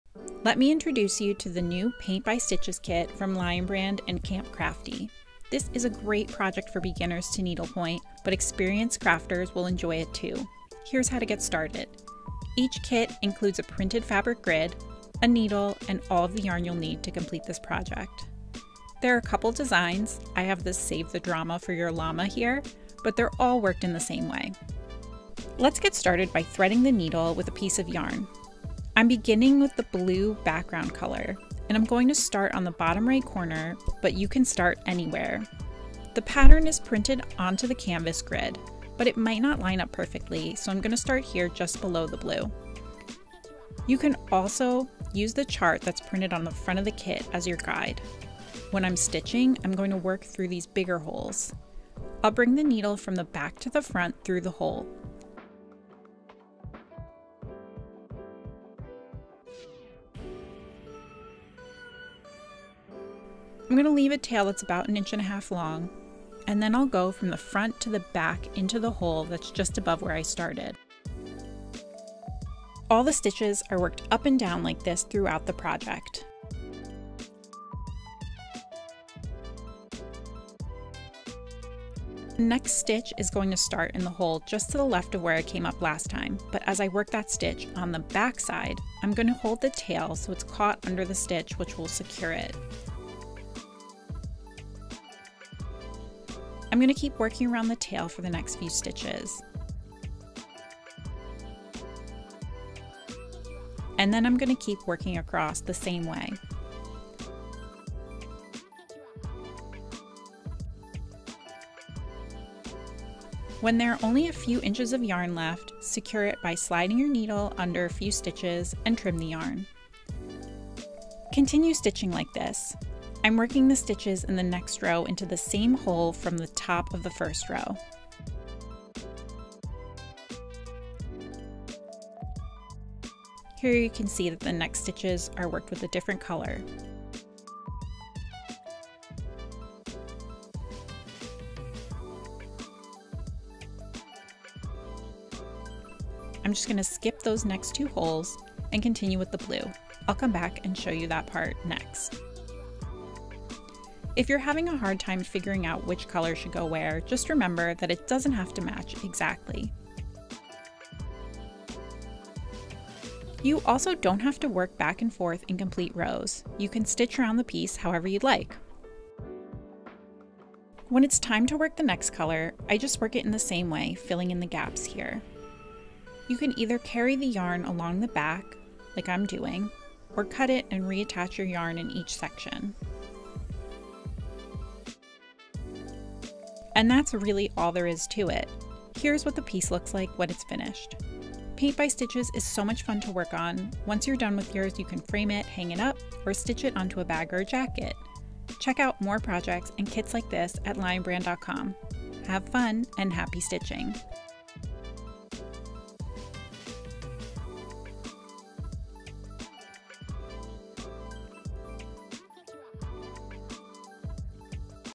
How to Needlepoint with Paint by Stitches!
Enjoy these fun and easy one-stitch needlework kits! Each kit contains yarn in assorted colors (100% acrylic), a needle, and an 8x8” pre-printed fabric grid (60% cotton, 40% polyester) in the chosen pattern – either an out of this world Rocket Ship or a no fuss, no drama Llama. Just stitch over the pre-printed design on the fabric grid to create these charmingly modern motifs!